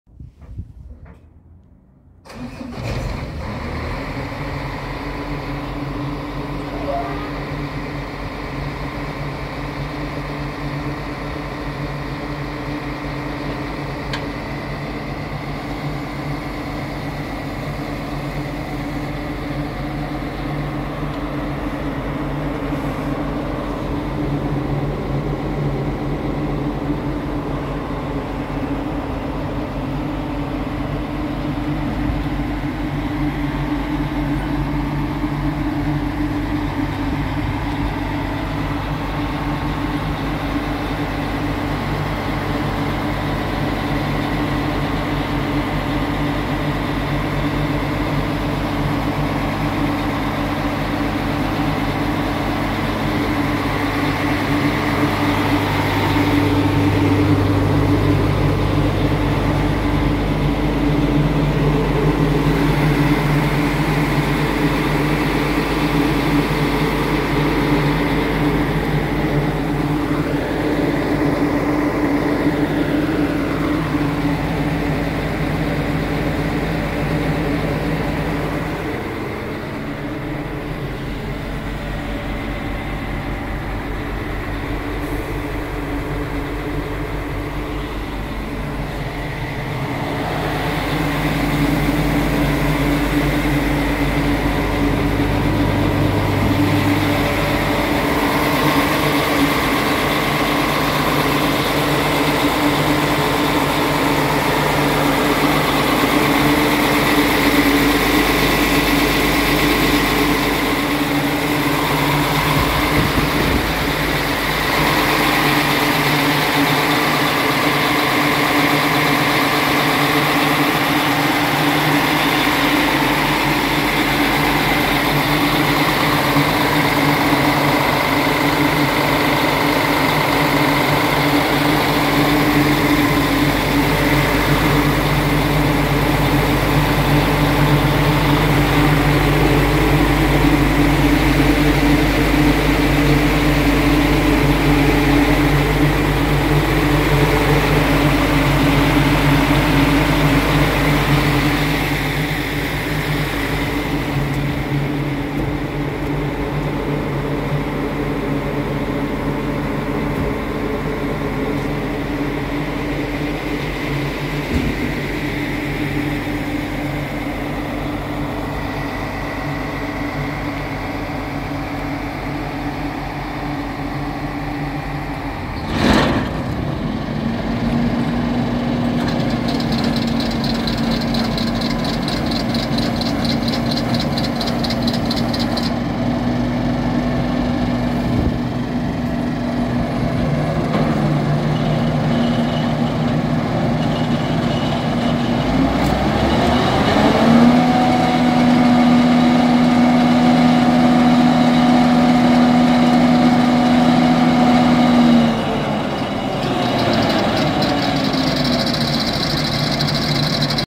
Claas Jaguar 840 forage harvester for sale @ A&M Machinery
2015 Claas Jaguar 840 4WD including Kemper 345 6 row corn header, 2706 engine hours and 2075 cutting hours, autolube, kernal processor (corn cracker) spout mounted and rear view camera, innoculant system factory installed, refrigurator underneed the buddy seat and air compressor.